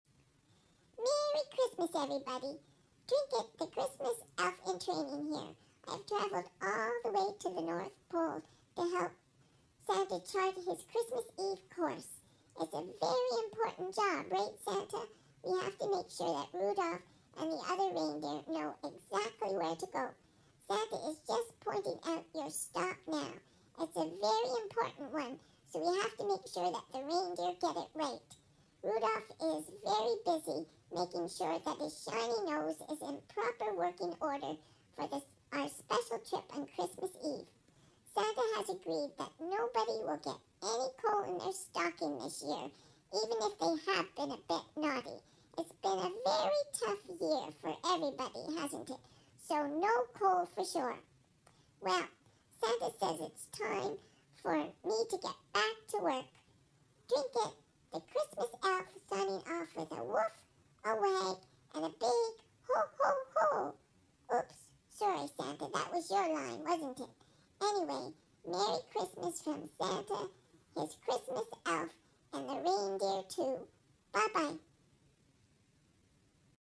Trinket the Christmas Elf
Trinket takes a break from helping out Santa, to send this special holiday message!